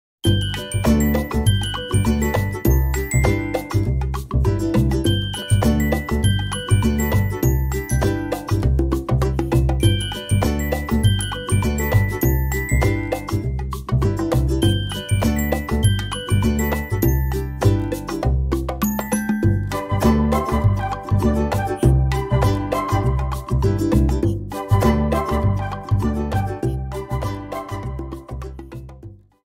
INVICO sorteó 90 viviendas en Ituzaingó
La gente colmó el Anfiteatro Salvador Sena de la ciudad.- 14 de febrero 2022.-